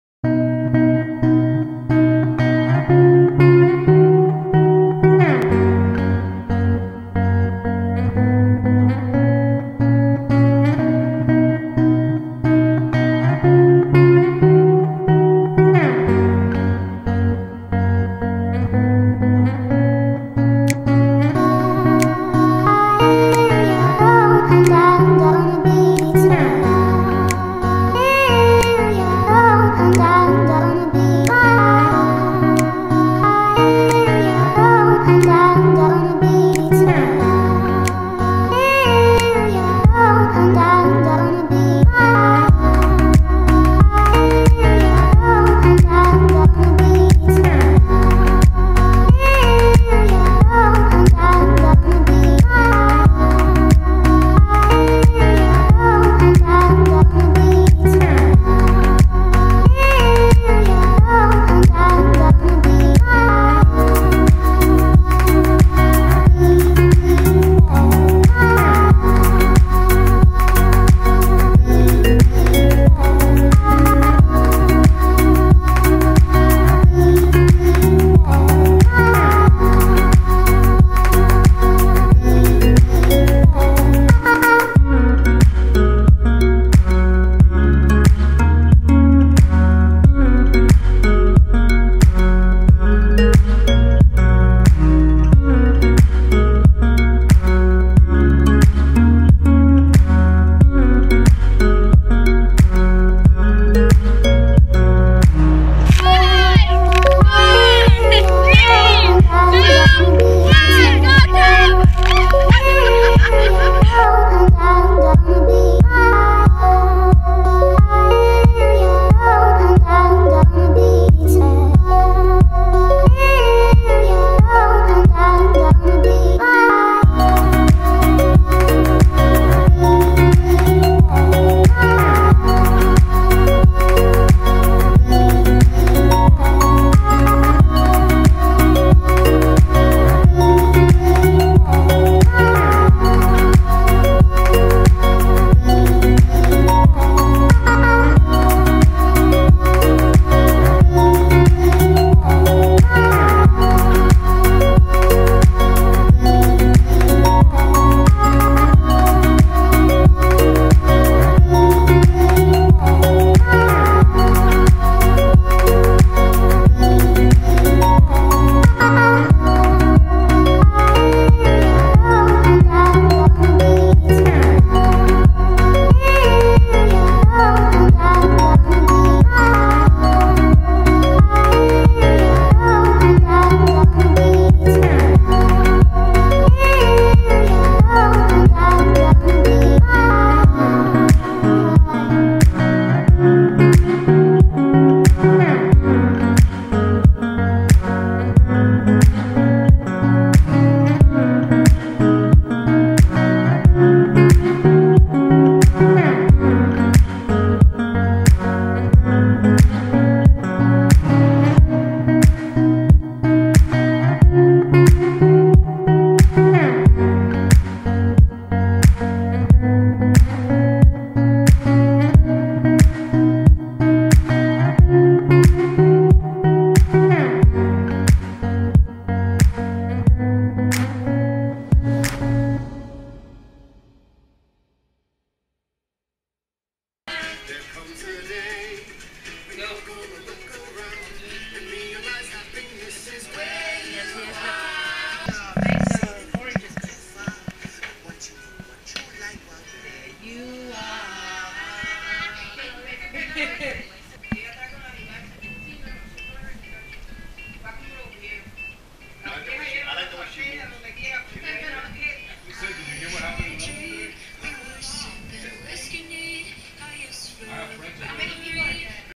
Marco Island June 2017 | GoPro Hero3
The cousins finally reunited again, plus Nick and Laura, and sadly missing Zack. New adventures this time with the Banana Boat!